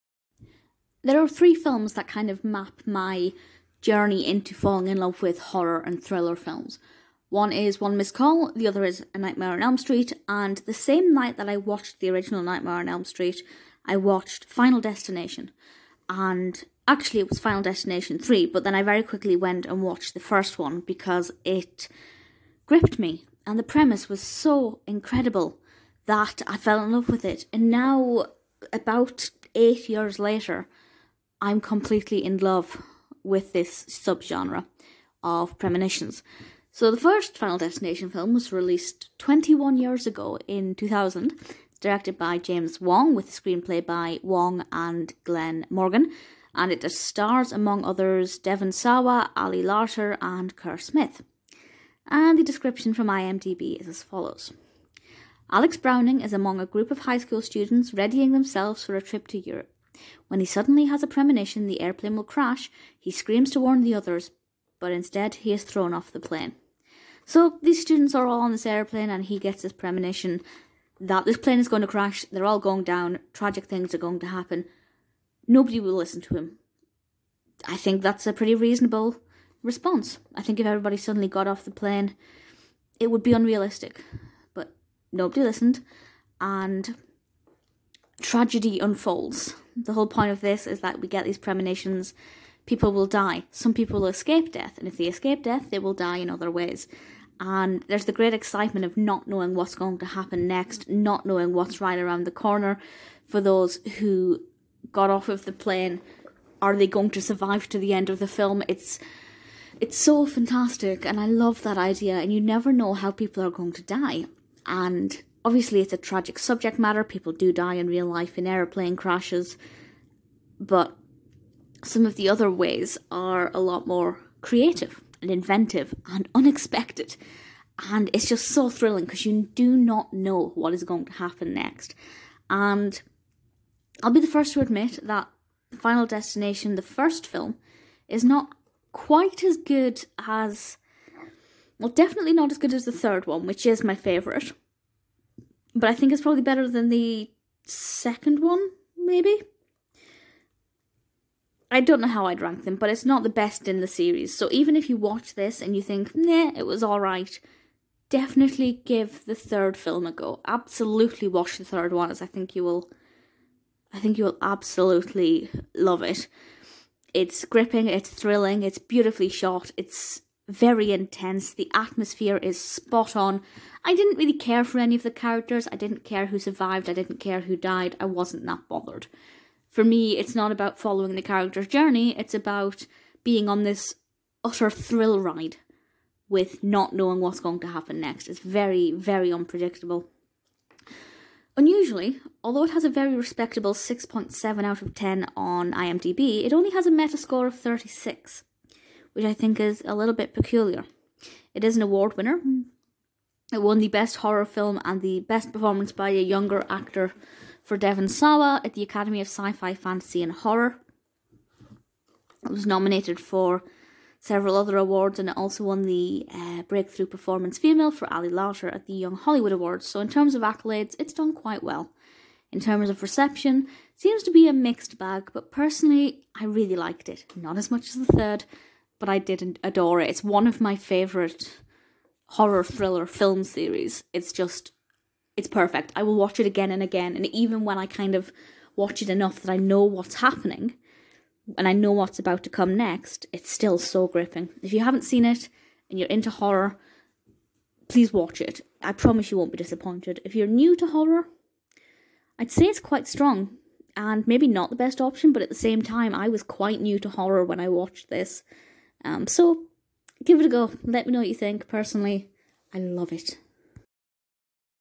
Final Destination 2000 Film | Devon Sawa, Ali Larter, Kerr Smith | Review
MY BOOKS What's your review of the 2000 thriller film Final Destination, directed by James Wong, and starring Devon Sawa, Ali Larter, and Kerr Smith? [OPEN FOR LINKS AND FACTS!] FinalDestination JamesWong DevonSawa Do you want to watch Final Destination? Did you enjoy the full movie or just parts of it? Scroll down for more information!

Amy McLean

ABOUT THE FIRST FINAL DESTINATION MOVIE

Final Destination plot on IMDb: "Alex Browning is among a group of high school students readying themselves for a trip to Europe. When he suddenly has a premonition their airplane will crash, he screams to warn the others but instead he is thrown off of the plane."

Directed by James Wong
Produced by Warren Zide, Craig Perry, Glen Morgan
Screenplay by Glen Morgan, James Wong, Jeffrey Reddick
Story by Jeffrey Reddick
Cinematography: Robert McLachlan
Edited by James Coblentz
Production companies: Zide/Perry Productions, Hard Eight Pictures
Distributed by New Line Cinema
Release date: March 17, 2000

Cast and Characters:
Devon Sawa as Alex Browning
Ali Larter as Clear Rivers
Kerr Smith as Carter Horton
Kristen Cloke as Valerie Lewton
Daniel Roebuck as Agent Weine
Roger Guenveur Smith as Agent Schrek
Chad E. Donella as Tod Waggner
Seann William Scott as Billy Hitchcock
Tony Todd as William Bludworth
Amanda Detmer as Terry Chaney
Brendan Fehr as George Waggner
Forbes Angus as Larry Murnau
Lisa Marie Caruk as Christa Marsh
Christine Chatelain as Blake Dreyer
Barbara Tyson as Barbara Browning
Robert Wisden as Ken Browning
P. Lynn Johnson as Mrs. Waggner
Larry Gilman as Mr. Waggner
Fred Keating as Howard Siegel